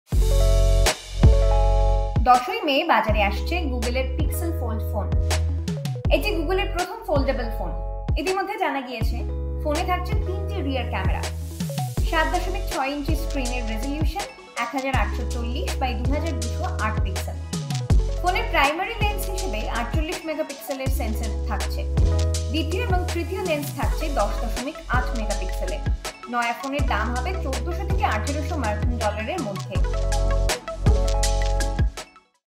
এ মাসেই বাজারে আসছে গুগলের প্রথম ফোল্ডিং ফোন, জেনে নিন ফিচার
টিজার থেকে ইতিমধ্যে জানা গিয়েছে যে পিক্সেল ফোল্ডে তিনটি রিয়ার ক্যামেরা থাকবে।